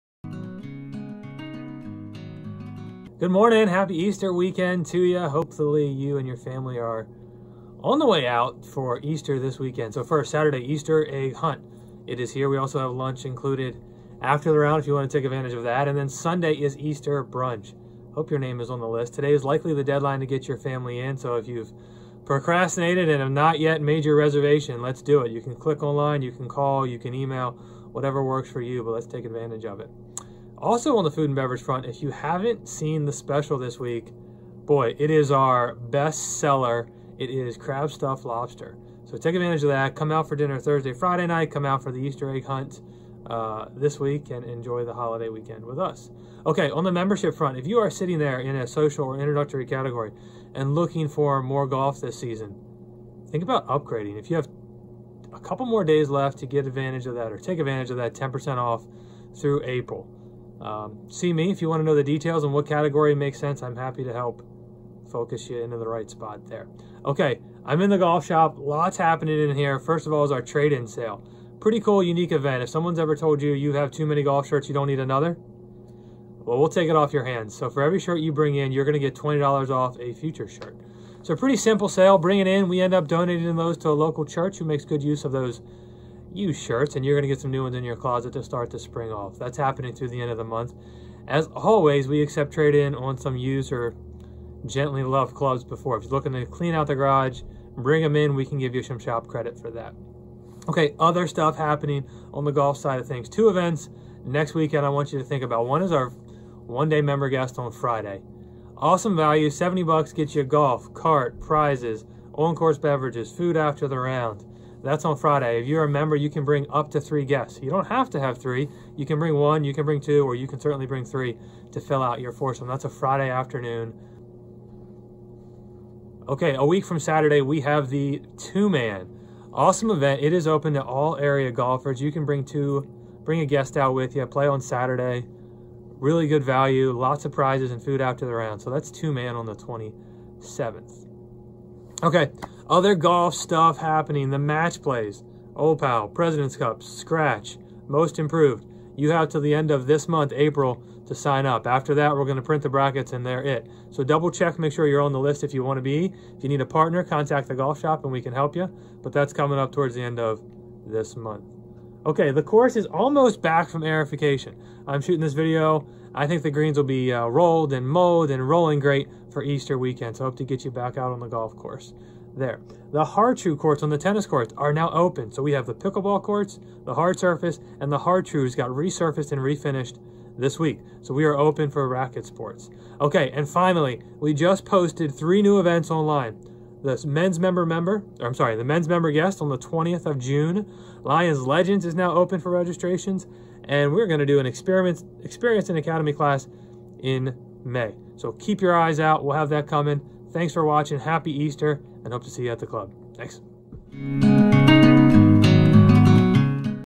Video Update April 18 2019
Weekly Update for April 18. Click to keep up to date with Chambersburg Country Club.

Highlights this week include the Easter Weekend activities & upcoming golf events.